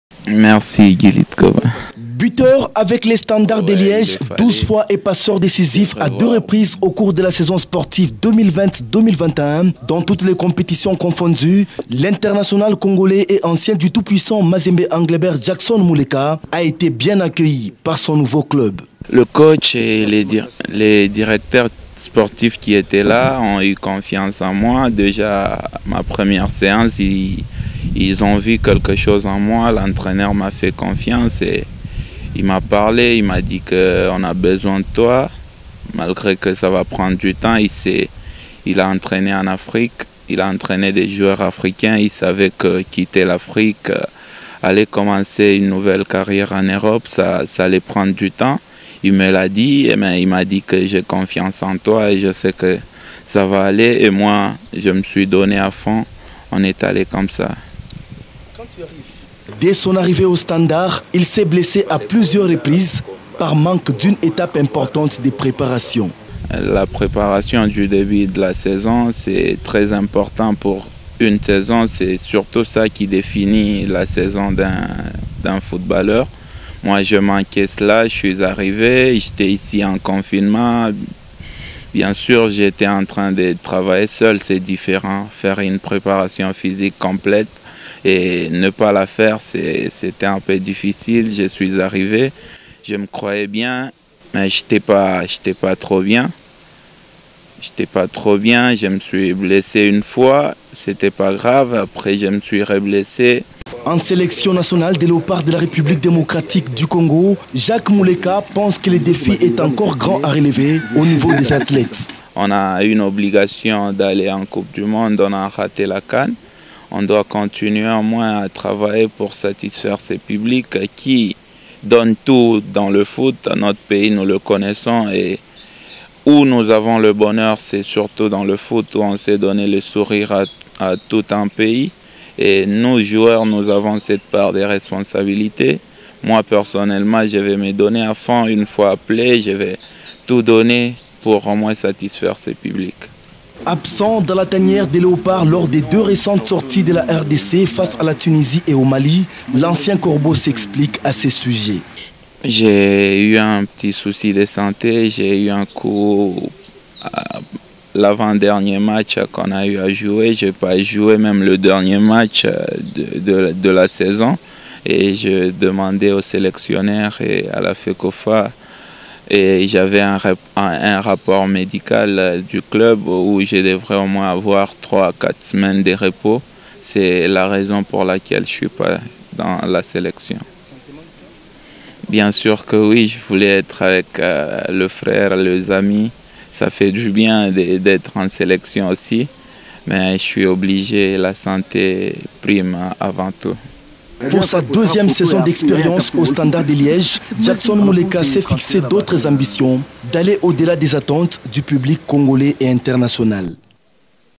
Jackson Muleka parle de ses blessures, en séjour à Lubumbashi
L'international congolais, parti chez Standard de Liège en Belgique, séjourne à Lubumbashi mi-juin 2021. Jackson Muleka parle de son engagement dans son nouveau club et de ses blessures.